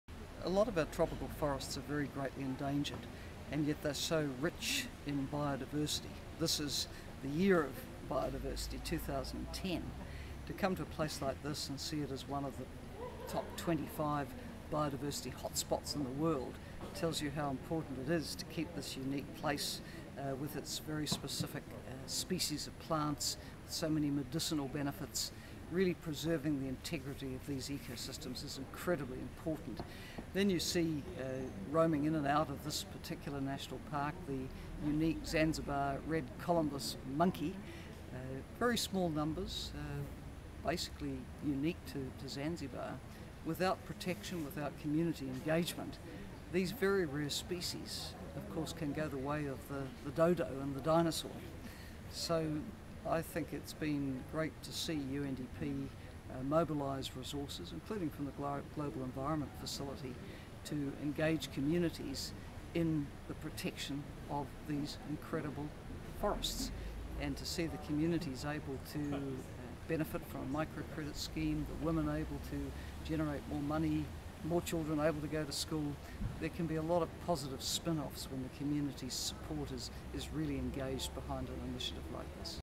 Helen Clark - Tanzania Biodiversity
Billions of people around the world depend on biodiversity because ecosystems are their primary source of food, fiber and medicines. Nevertheless, this significant contribution of biodiversity to human livelihoods is not valued or recognized in markets. As a consequence, biodiversity is being lost at an unparalleled pace since natural resources are used without considering their other values. The Millennium Ecosystem Assessment (2005) concluded that almost 60% of the ecosystem services that support life and make a direct contribution to human wellbeing - such as provision of freshwater, pollination and the regulation of regional climate - are being destabilized.
In this video, Helen Clark visits Jozani - Chwaka Bay Conversation Area in Tanzania, which is an example of a sustainable, community based conservation and management of significant biodiversity. This natural protected forest reserve covers about 30km² about 23 miles South of Zanzibar Town. The coral rag zone is habitat for wildlife species of national and international significance including the endemic and near extinct Zanzibar Leopard, the Red Colobus Monkey and the Ader Duiker. Jozani forest is the only area where these particular species could be preserved in-situ. The forest harbours five important vegetation types: the ground water forest; the coral rag forest ; salt marsh; mangrove forest; and sea grass beds.
UNDP supported the creation of a National Park - which protects the Jozani Forest  - and worked with the government in Zanzibar to enact policies and legislative processes for the conservation of its biodiversity, which is key to sustaining the livelihoods of the local communities. The Jozani Forest is now managed by a community-led organization that gives 65 percent of the proceeds from managing the park back to the local community. Up to 90 percent of the associations staff are locals from the villages situated within the national park. The project helped to create new economic opportunities with a special focus on wimen.